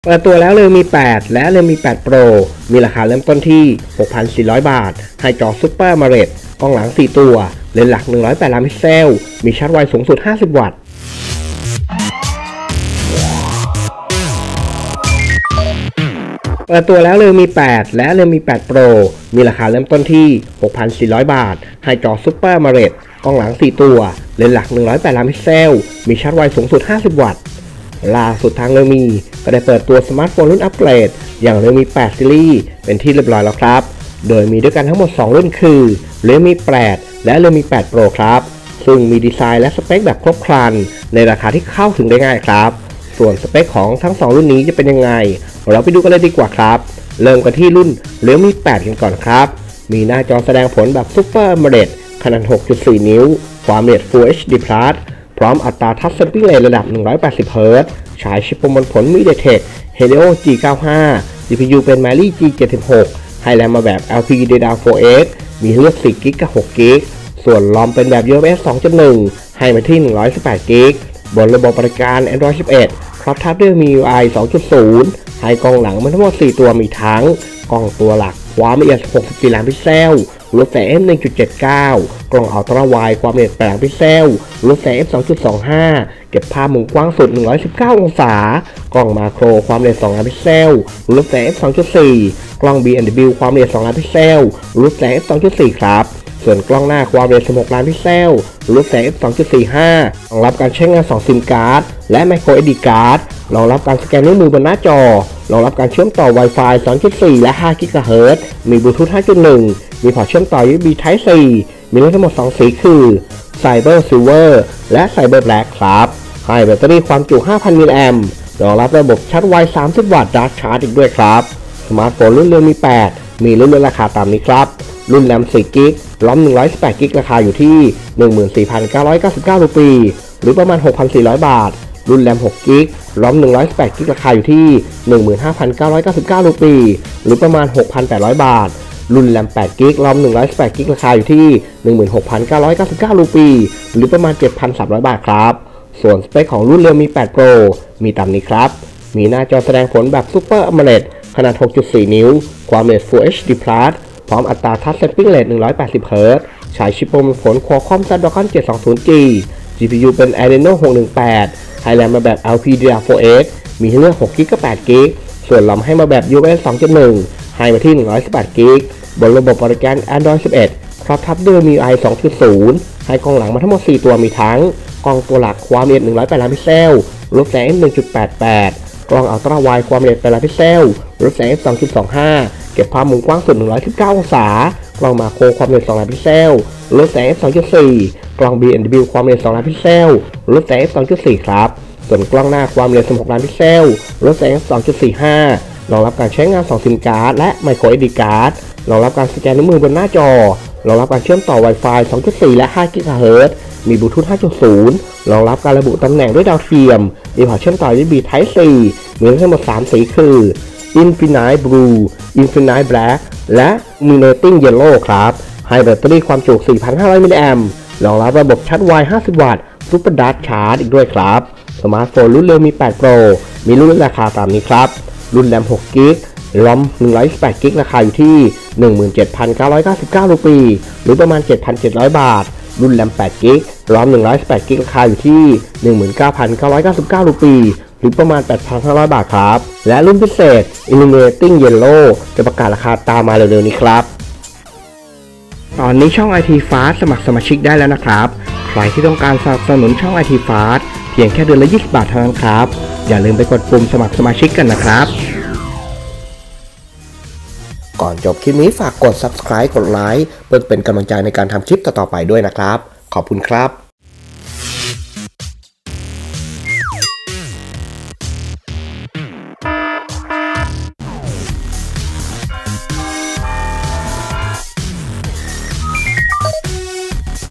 เปิดตัวแล้ว realme 8 และ realme 8 Pro มีราคาเริ่มที่ 6,400 บาท
เปิดตัวแล้ว realme 8 และ realme 8 Pro มีราคาเริ่มที่ 6,400 บาท ให้จอ Super AMOLED  กล้องหลัง 4 ตัว เลนส์หลัก108MP มีชาร์จไวสูงสุด 50W

ที่มา realme (India) - Dare to Leap

ช่อง Youtube IT FAST (ข่าวสารไอที สมาร์ทโฟน)